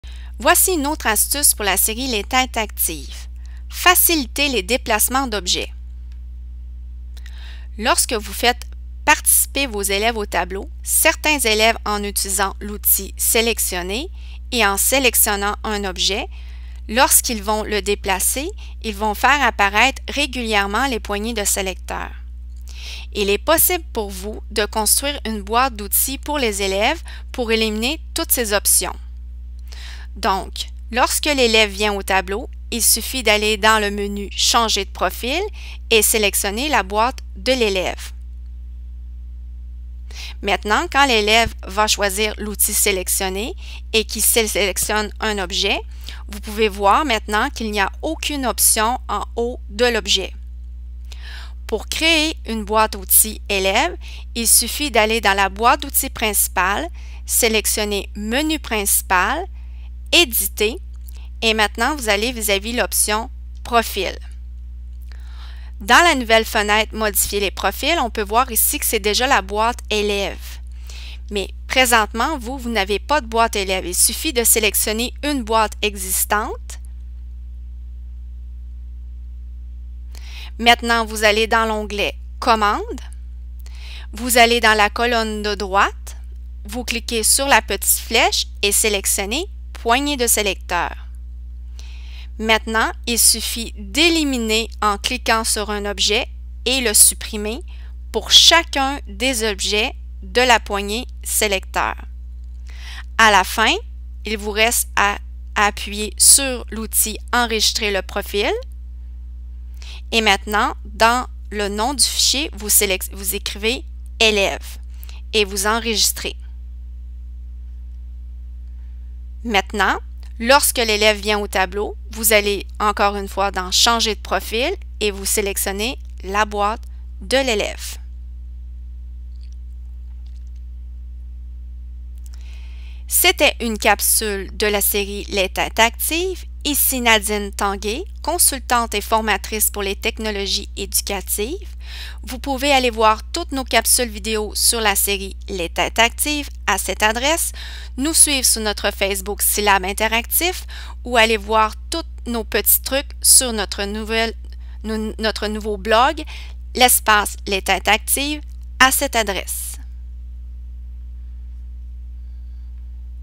Facilitez les déplacements d'objets
Un petit truc pour aider la manipulation d'objets sur le TNI avec le logiciel ActivInspire.

Cliquez sur ce lien pour aller sur la Chaîne Les TêtesActiv pour voir toutes nos capsules

Nadine Tanguay
Consultante et formatrice
pour les technologies éducatives